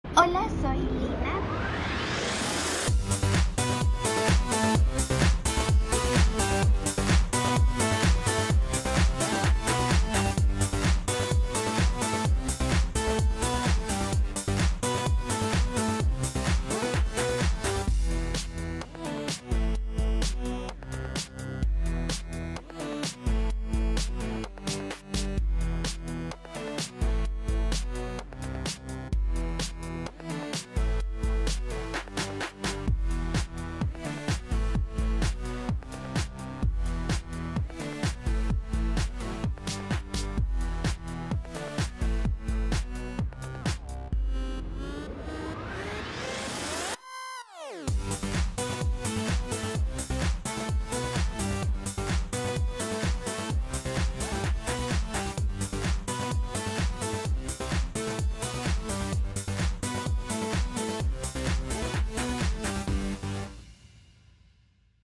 ❀❀ BIENVENIDOS A MI CANAL ❀❀
Hola soy Lyna, me conocen como 100prekool y es que mi canal se trata de ¿Como ser cool? por eso aquí podrás ver tutoriales de belleza como peinados, maquillajes, recetas de cocina, DIY, y muchas cosas mas.
Mi snapchat: lyna-100prekool